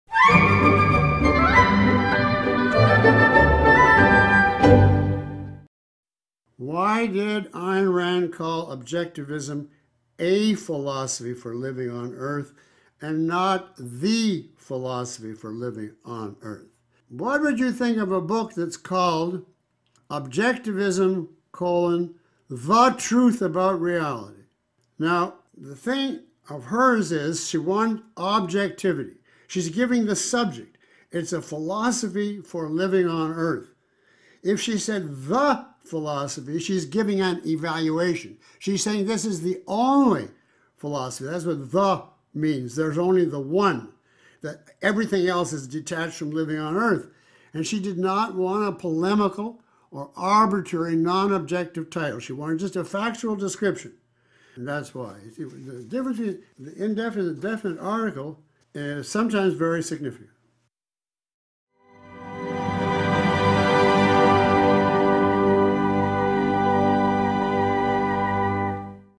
Por qué *una* filosofía para vivir en la Tierra
¿Por qué se refirió Ayn Rand a Objetivismo como "una" filosofía, en vez de "la" filosofía para vivir en la Tierra? Al fin y al cabo, es la única filosofía que de hecho es válida y funciona, ¿no?